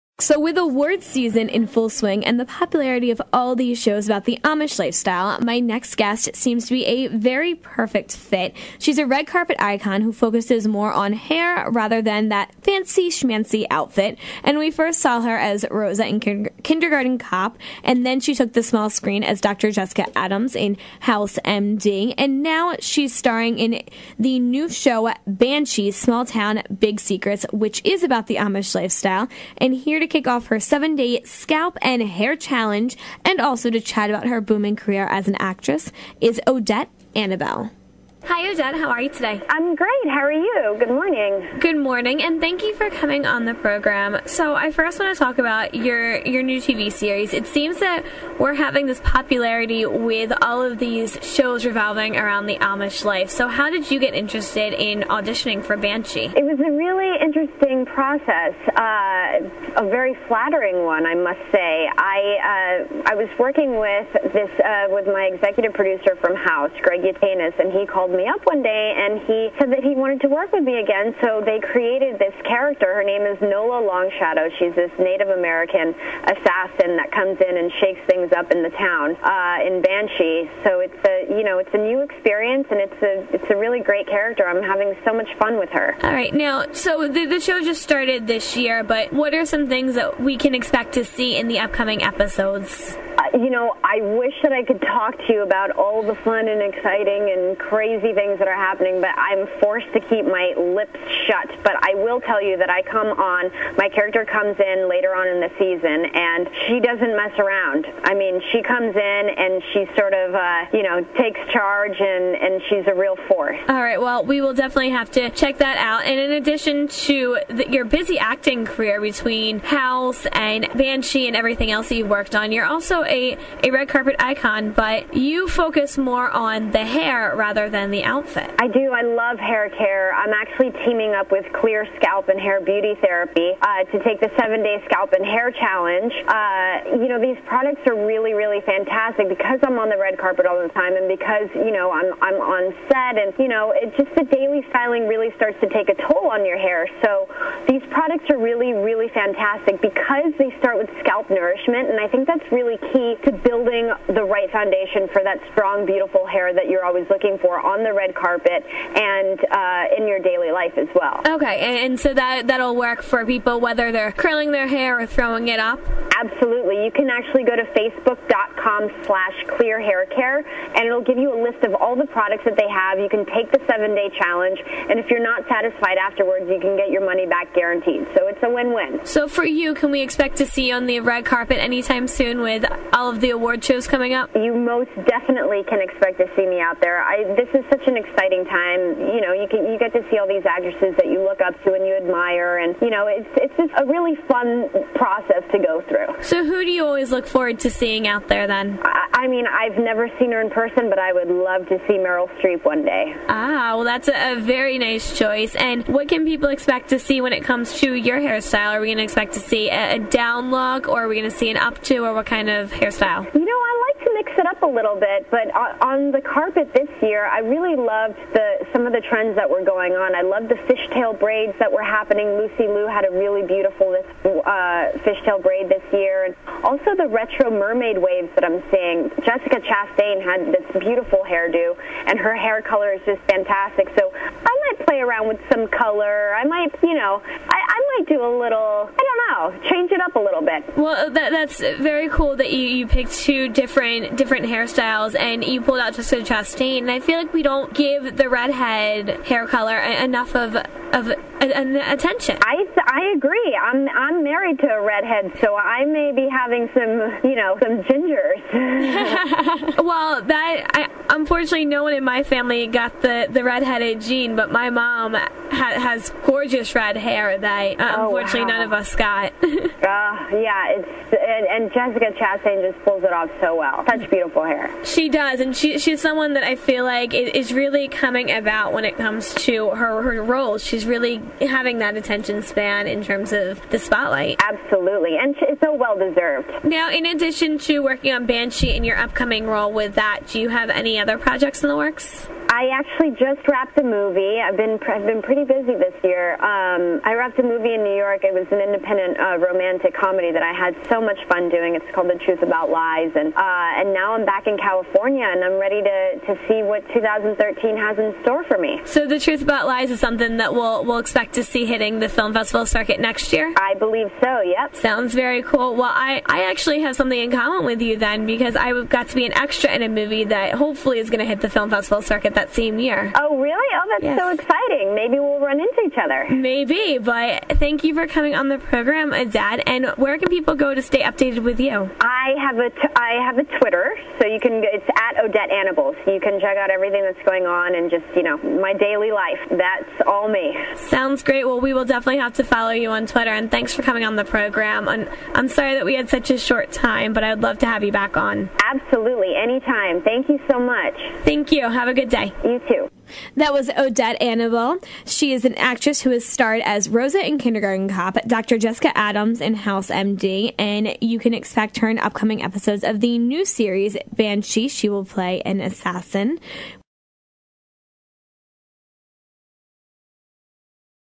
The Brynn Project: Odette Annable
Brynn McKenry talks with Odette Annable, actress in the new show Banshee, about her red carpet hair icon status and how to get flawless hair on this edition of The Brynn Project.